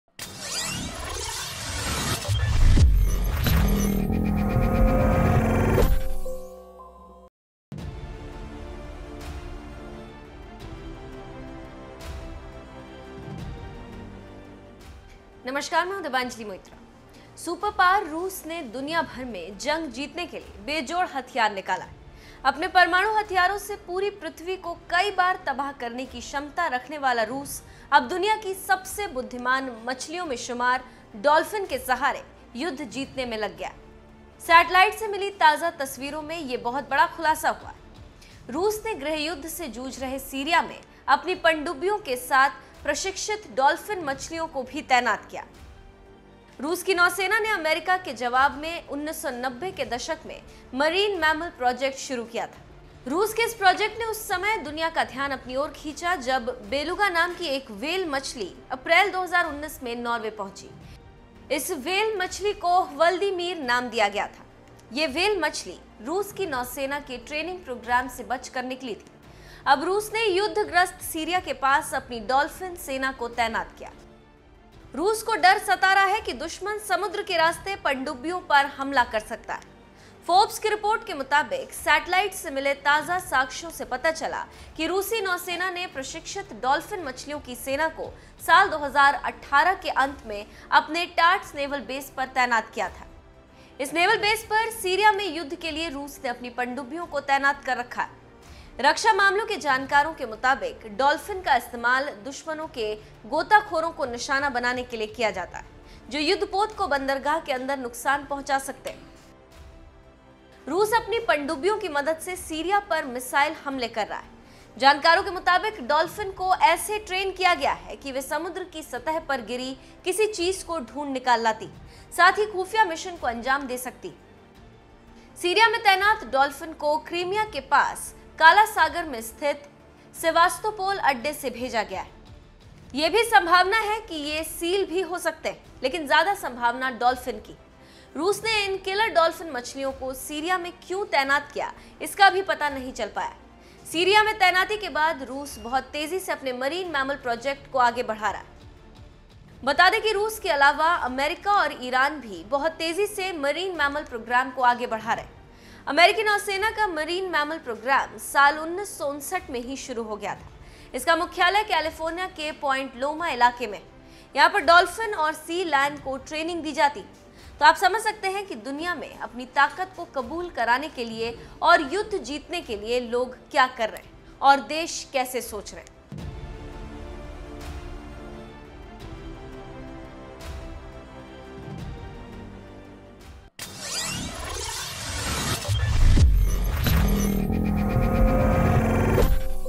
सीरिया में रूस की मछलियों की सेना तबाही मचा रही है
सेटलाइट से मिली ताजा तस्‍वीरों में यह बहुत बड़ा खुलासा हुआ है…रूस ने गृहयुद्ध से जूझ रहे सीरिया में अपनी पनडुब्बियों के साथ प्रशिक्षित डॉल्फिन मछलियों को भी तैनात किया है…रूस की नौसेना ने अमेरिका के जवाब में 1990 के दशक में मरीन मैमल प्रॉजेक्‍ट शुरू किया था…